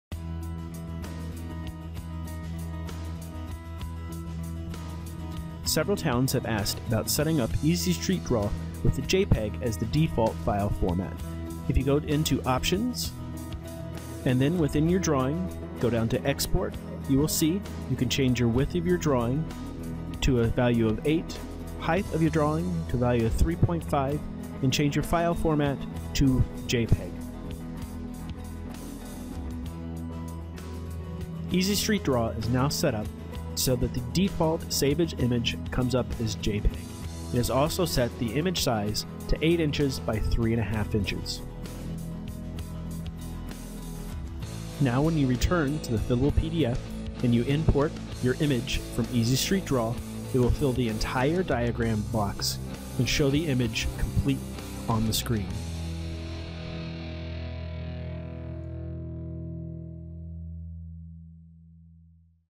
How to Setup Easy Street Draw with JPEG as the default image format
This video describes how to setup the save as image in ESD with a default file format or JPEG.  It also shows how to set your image size to fill the entire diagram box on the PR-1